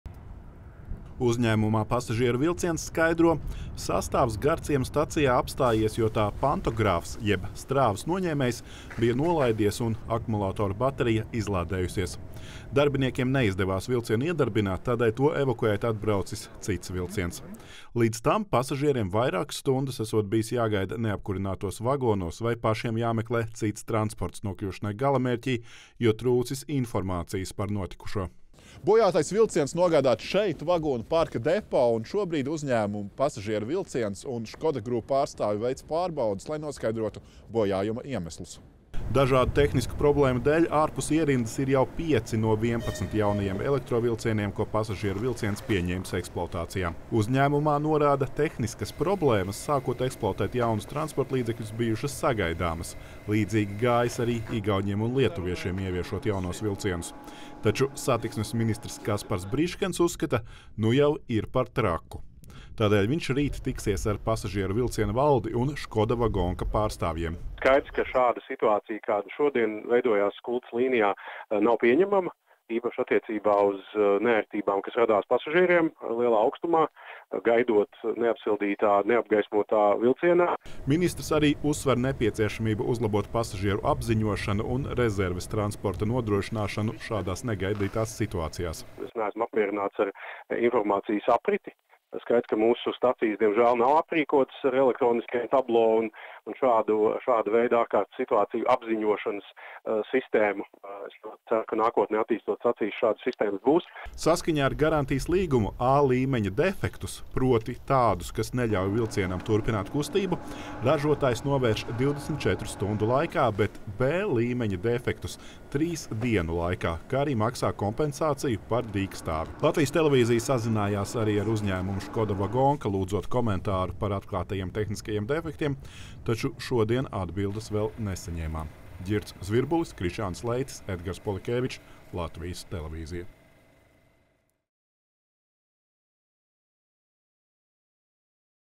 Jauni vilcieni, jaunas problēmas
“Ierados stacijā, teicu runu, bet vilcienu nevarēja iedarbināt,” šādu politiķa murgu pirms jauno vilcienu pirmā reisa esot redzējis satiksmes ministrs Kaspars Briškens (P). Atklāšanas dienā gan viss noritēja gludi, bet ministra murgs piepildās tagad, un to uz savas ādas izjūt arvien vairāk pasažieru.

Šodien jau kārtējais jaunais vilciens tehnisku iemeslu dēļ izstājies no ierindas. Maršrutā Saulkrasti–Rīga kursējošais sastāvs šorīt apstājās Garciema stacijā un vairs nebija iedarbināms. Tādēļ tika atcelti vai kavējās arī vairāki citi reisi Skultes līnijā, un pasažieriem nācās gaidīt vai meklēt citus veidus, lai nokļūtu galamērķī.